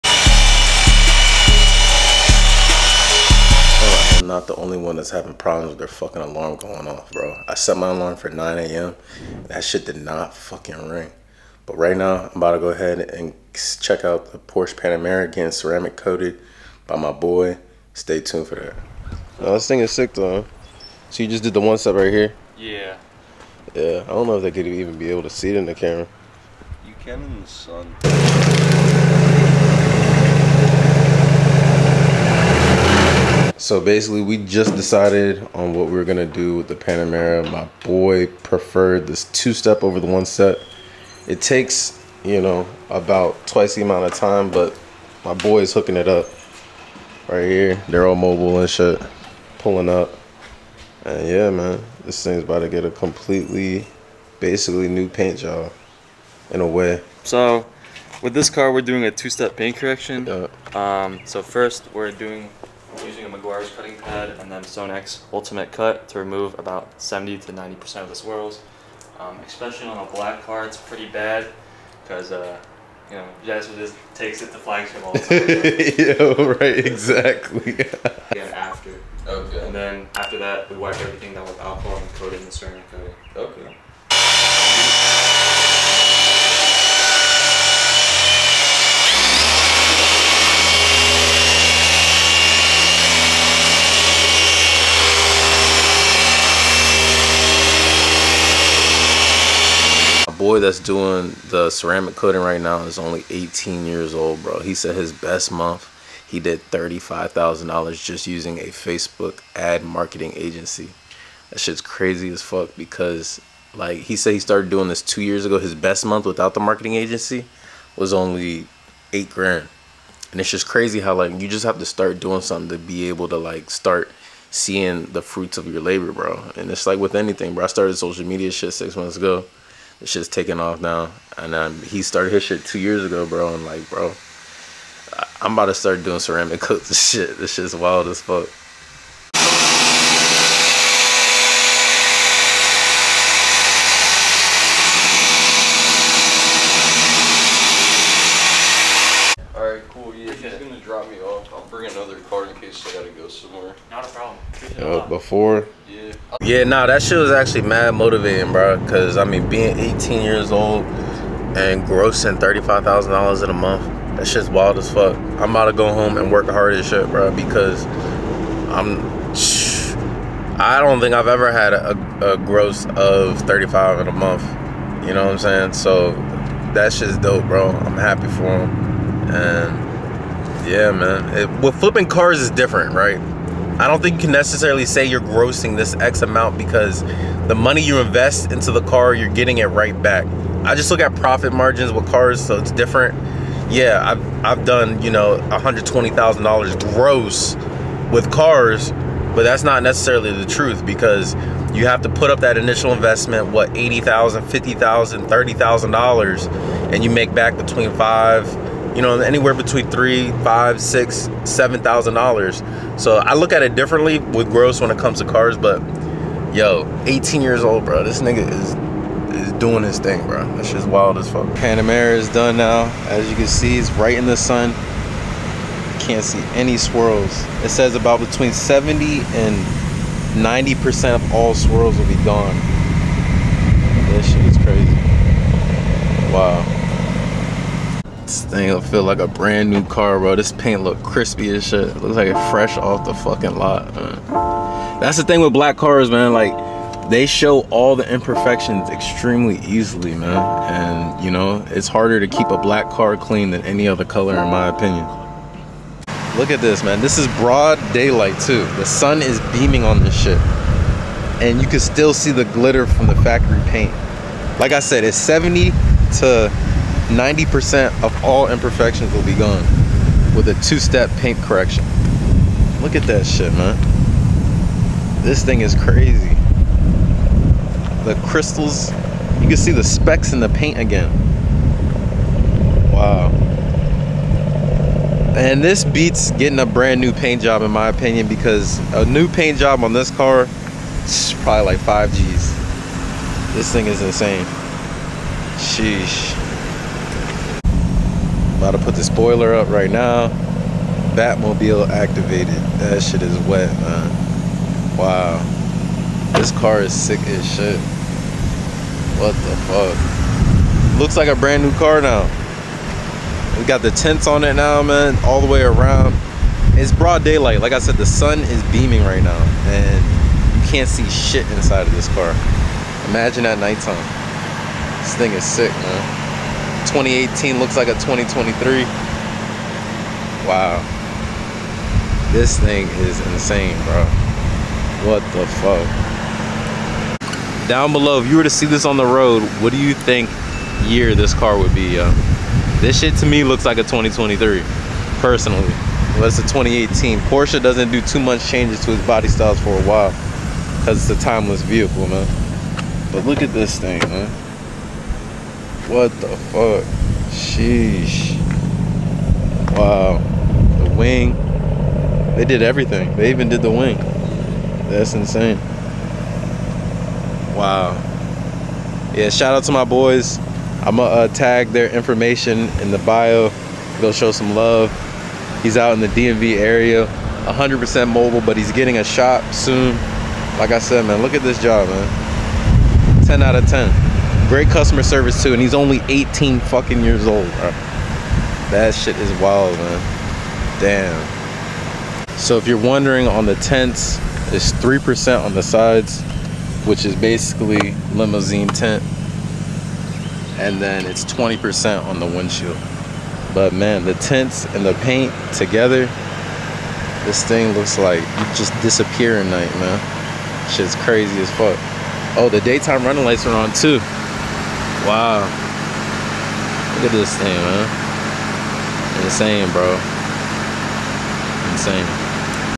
PANAMERA TURBO GETS A NEW LOOK?!?!
Shoutout to my boy over at deluxepaintprotection on instagram. They did a quick and efficient job on the 2018 Porsche Panamera Turbo. Looks like a brand new car from factory.